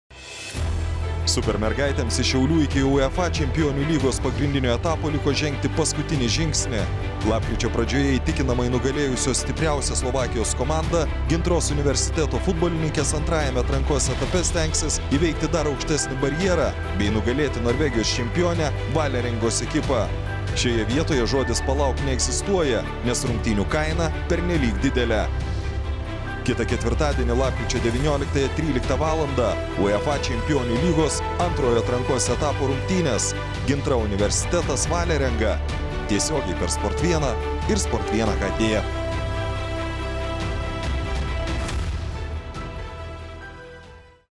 UEFA Čempionių lygos atranka: Gintra - Valerenga anonsas SPORT1HD
Stebėkite tiesioginę rungtynių transliaciją iš Telšių stadiono - lapkričio 19 d. 13 val.